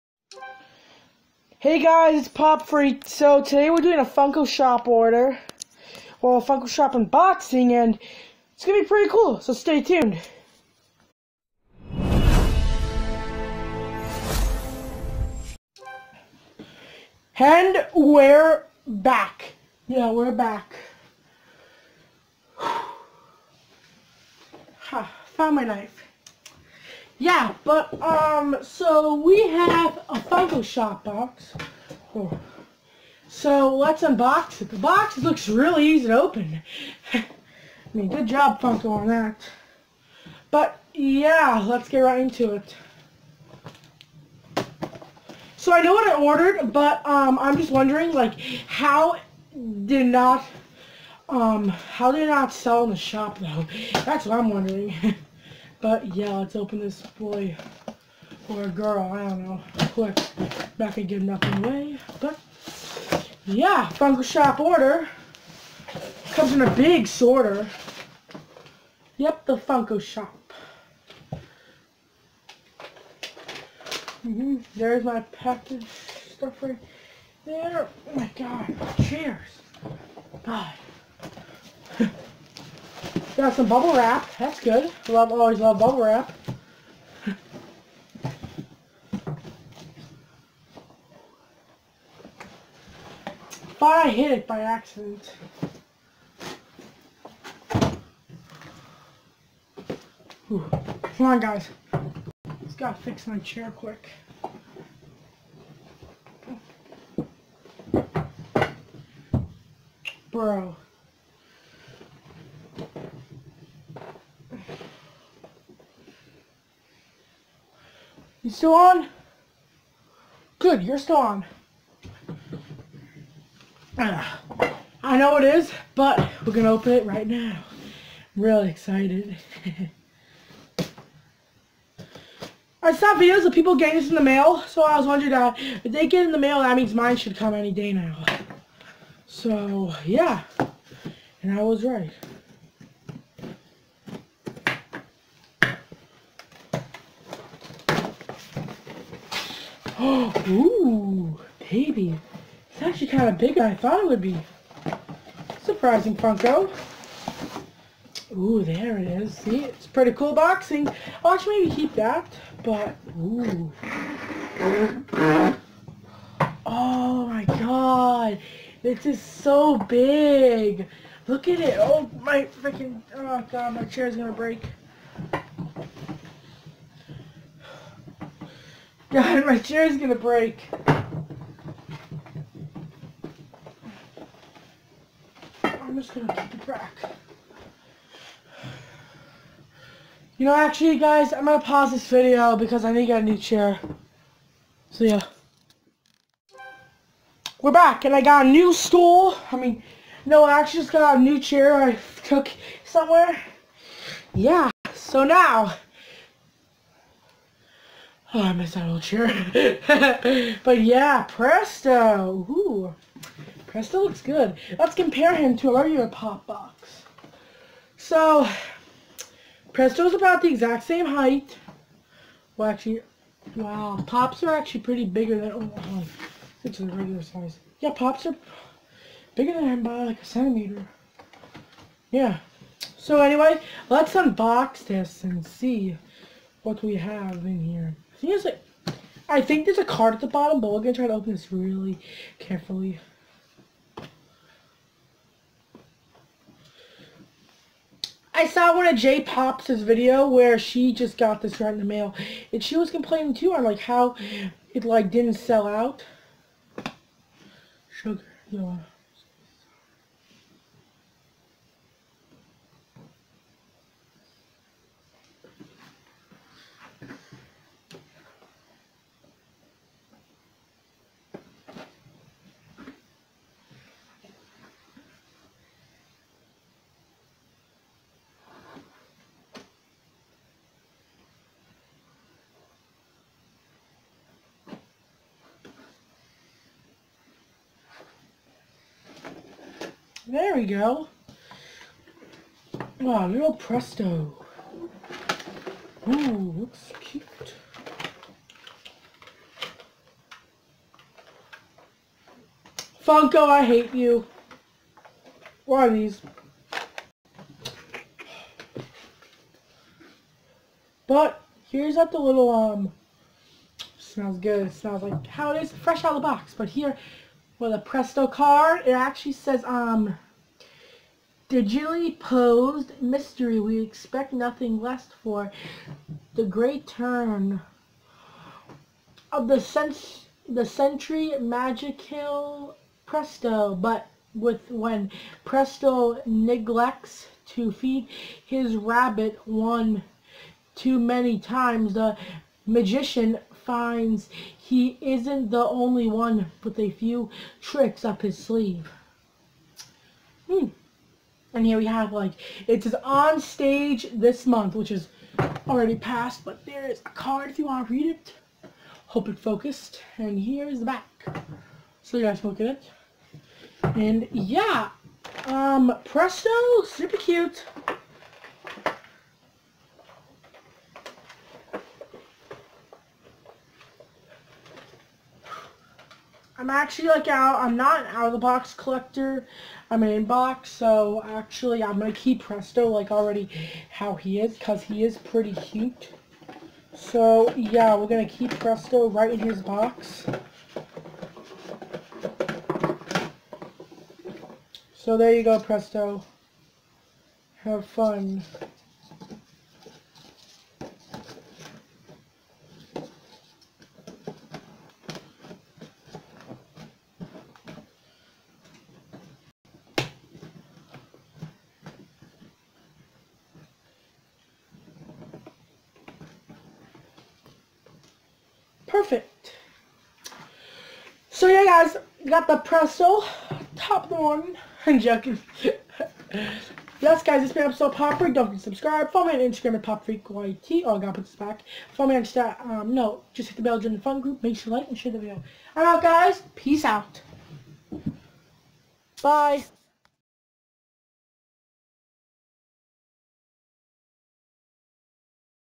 Mail call #9 it's so cute
Today we do a funko shop order and it's a good one so get ready and as always make sure to subscribe and hit the bell to join the funko group